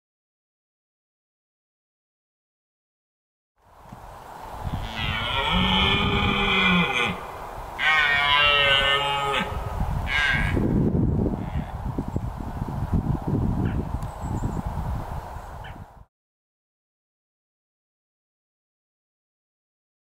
The roaring stag
Male Stag roaring during rutting season. turn volume up to hear him ROAR!